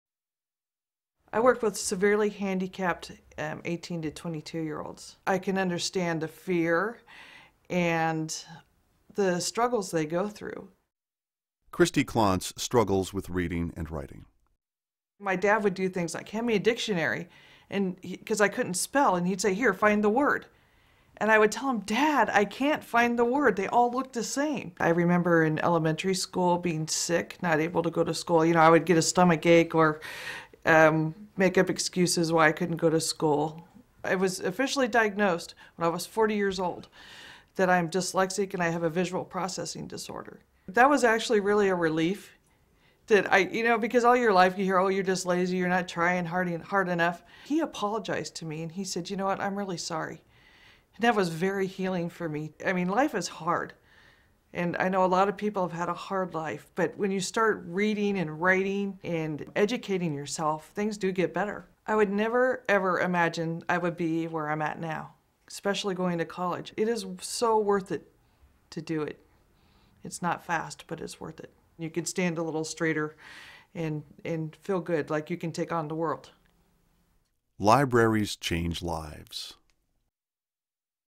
Kristi: A Learner's Perspective
An adult learner shares the impact of low literacy on her life.